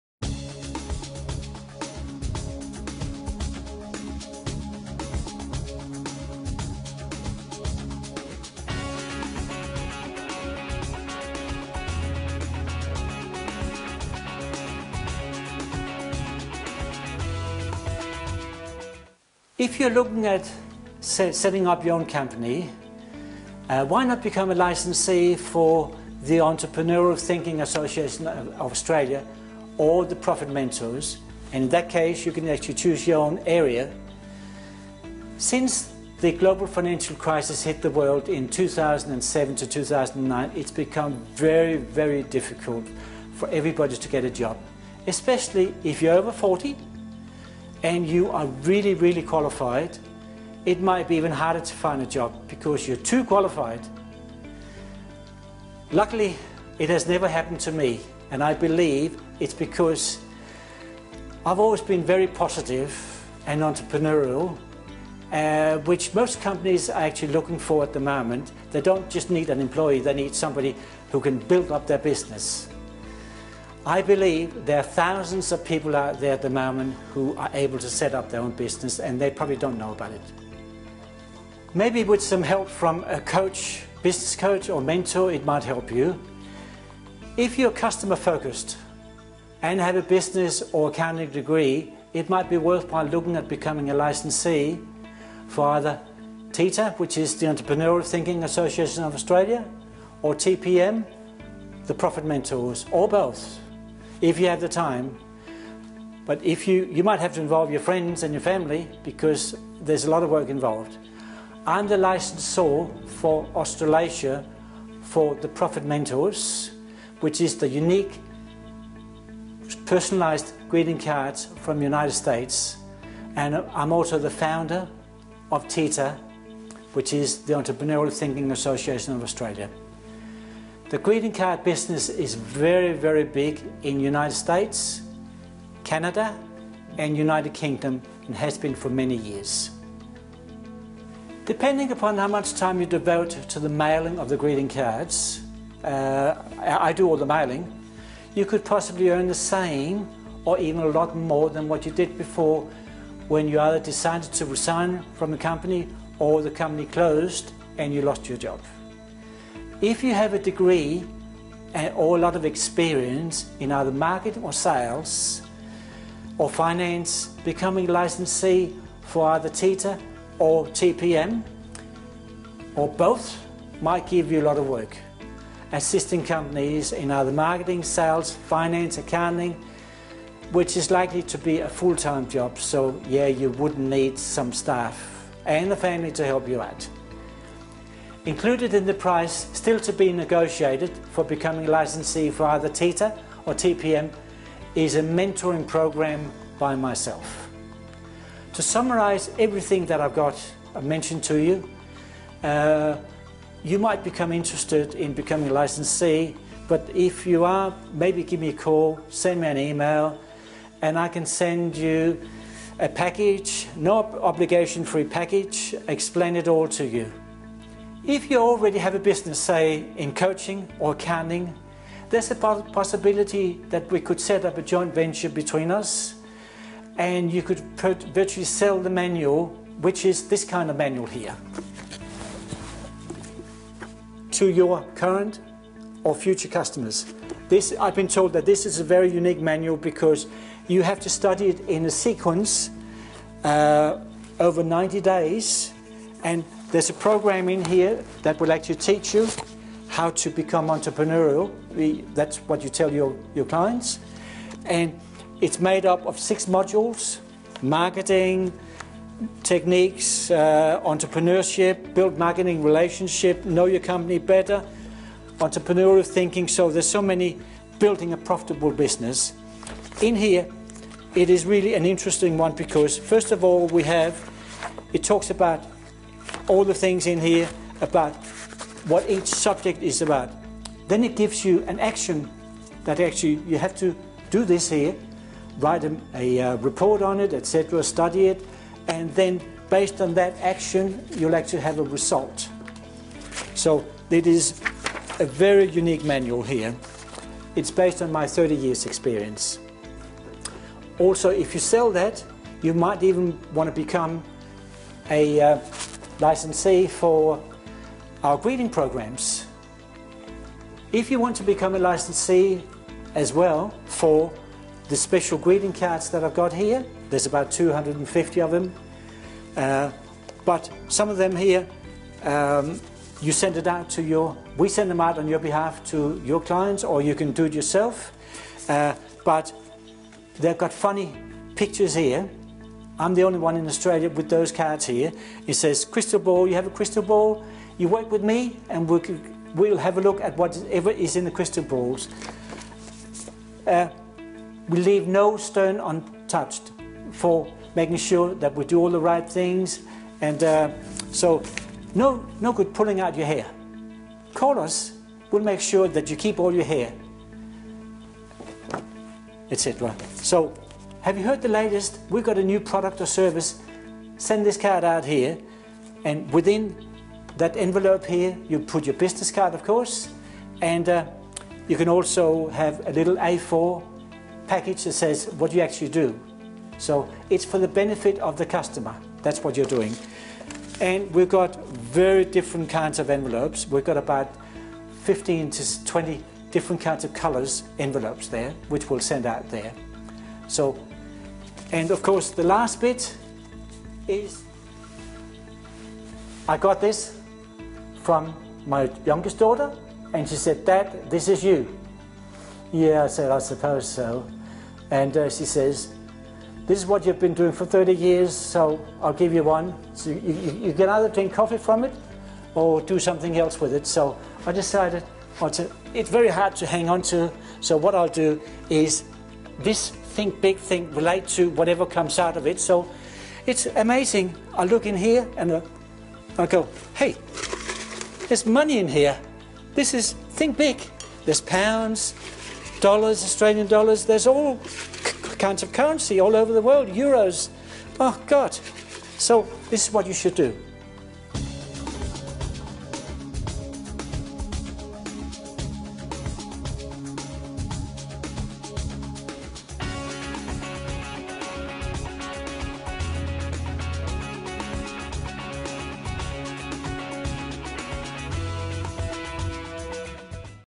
Creating your own opportunity as a licencee
Ole Nielsen from The Entrepreneurial Thinking Association of Australia